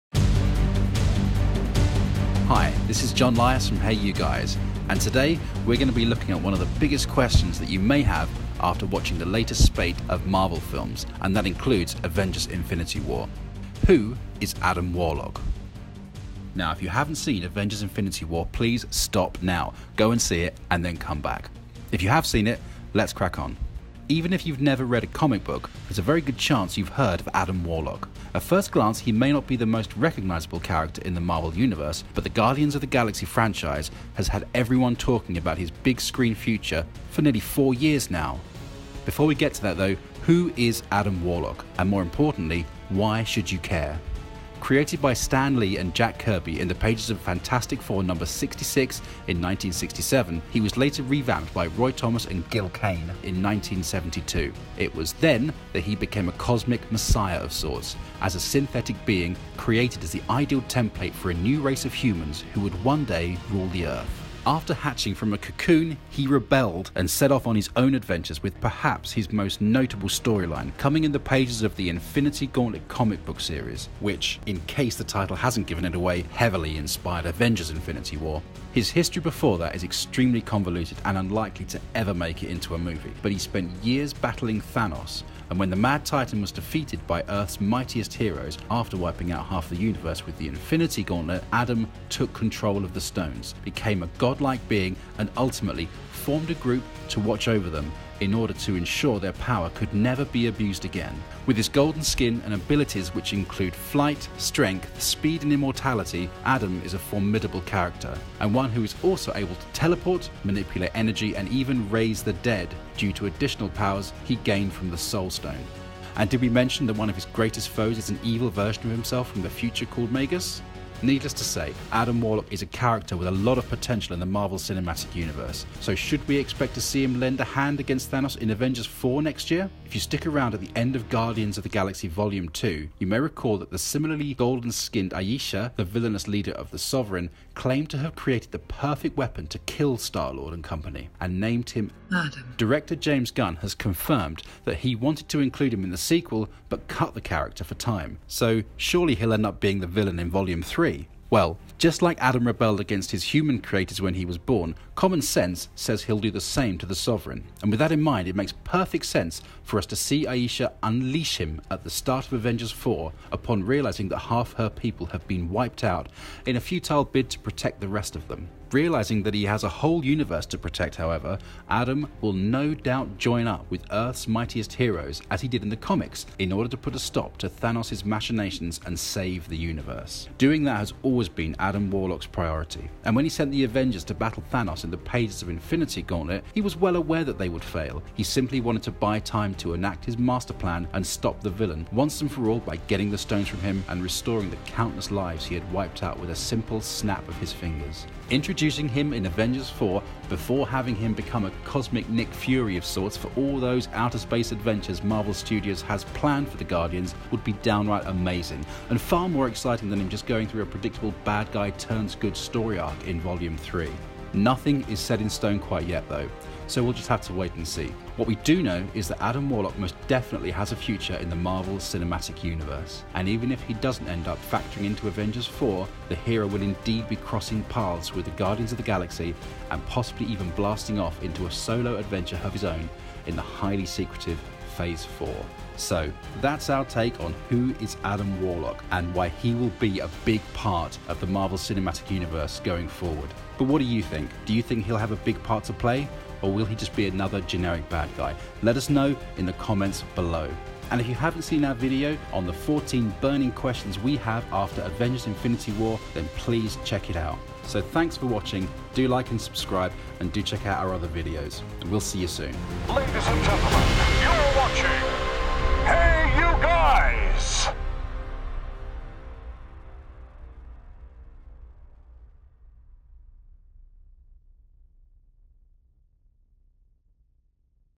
Who is Adam Warlock and how does he connect to The Avengers?
In our latest video about the ongoing Marvel Cinematic Universe we ask the question Who is Adam Warlock?

Following the Sovereign's tease in the post credits sequence of Guardians of the Galaxy Vol. 2 we expect the MCU to see its fair share of Adam Warlock before too long. We look at how he could appear in Avengers 4 following the incendiary events of Avengers Infinity War. He spent years battling Thanos and when the Mad Titan was defeated by Earth's Mightiest Heroes - after wiping out half the universe with the Infinity Gauntlet.
Adam Warlock most definitely has a future in the Marvel Cinematic Universe and even if he doesn't end up factoring into Avengers 4, the hero will indeed be crossing paths with the Guardians of the Galaxy and possibly even blasting off into a solo adventure of his own in the highly secretive Phase 4.